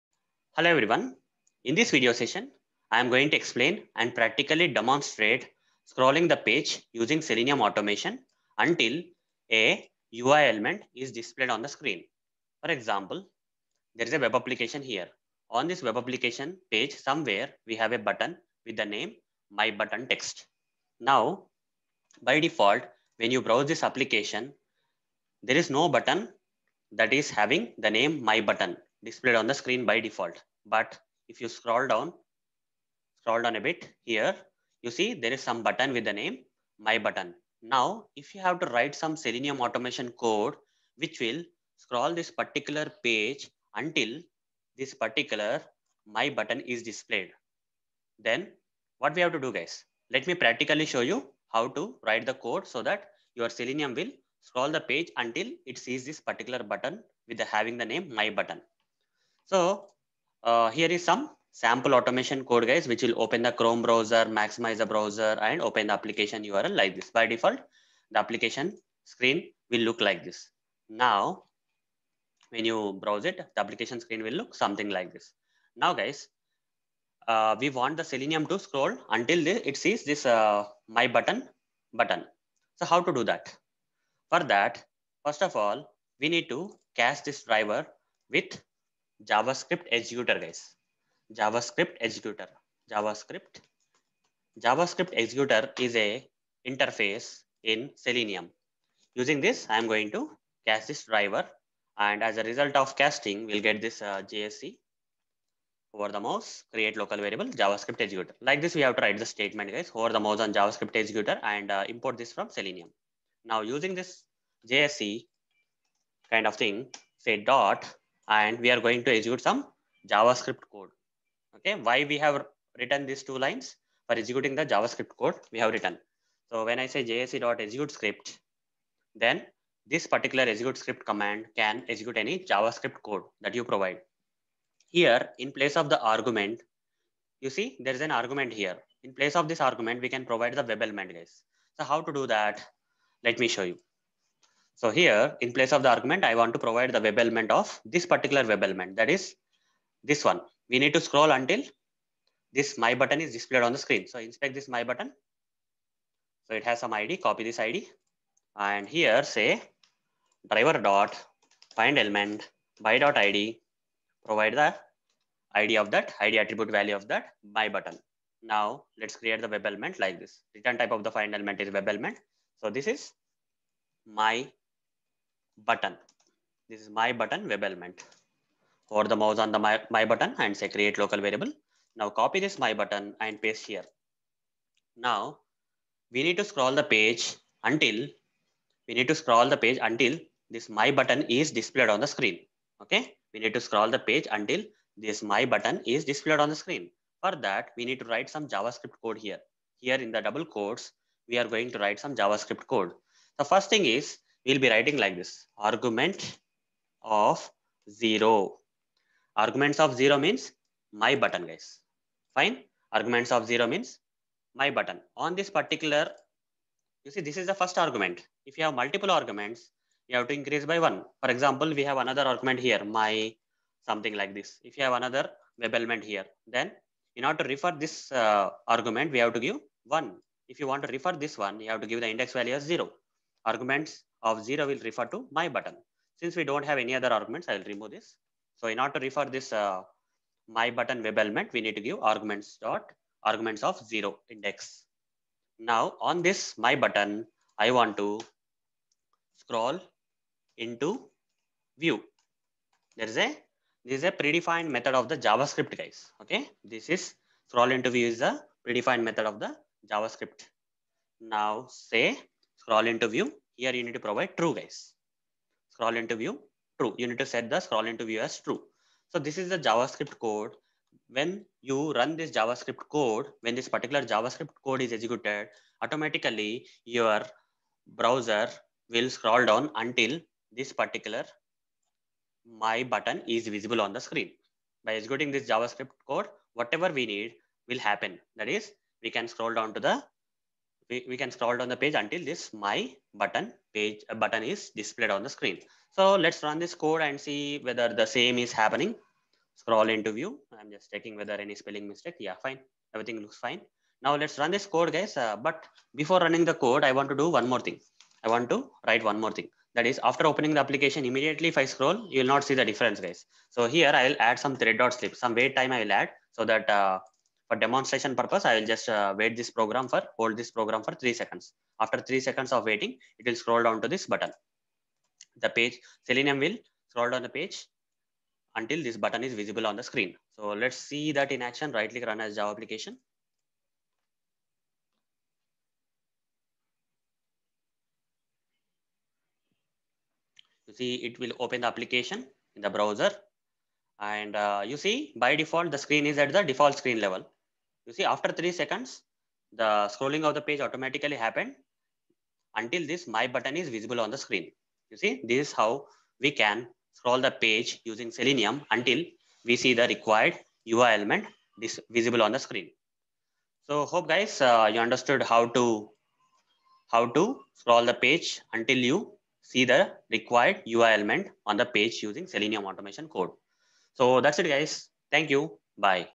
Scrolling lengthy page until the UI element is displayed on the screen using Selenium
In this video, I have explained and practically demonstrated scrolling a lengthy page until the required UI element is displayed on the system screen.